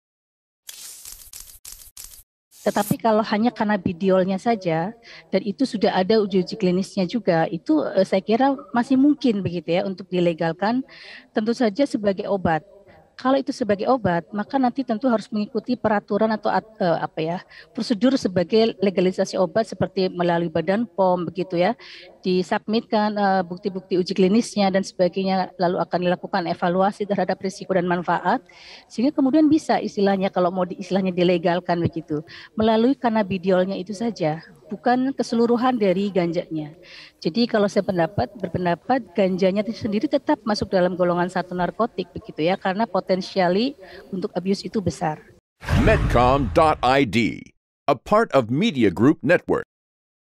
Zat Aktif Cannabidiol dalam Ganja Masih Mungkin Jadi Obat di Indonesia?
Menurut guru besar fakultas farmasi UGM, Zullies Ikewati, zat aktif dalam ganja, yaitu Cannabidiol masih mungkin untuk dijadikan obat yang dapat dilegalkan di Indonesia. Zukkues juga menegaskan bahwa hal tersebut didasari oleh uji-uji klinis yang beredar di luar negeri.
Namun, legalisasi obat tersebut tentunya perlu melalui BPOM dengan menunjukkan uji-uji klinis serta evaluasi lebih lanjut terkait risiko dan manfaat zat aktif Cannabidiol, bukan ganjanya.